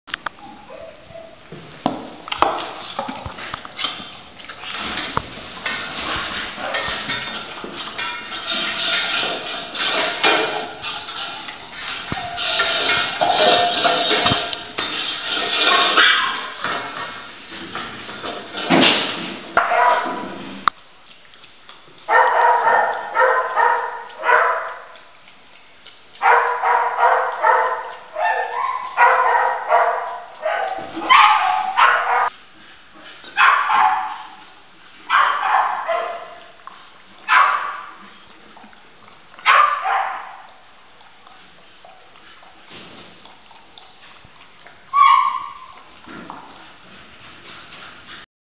Starving dogs at west Hull home (RSPCA video)
Two Staffordshire bull terriers left starving at a west Hull home are shown desperate for food and water in this RSPCA video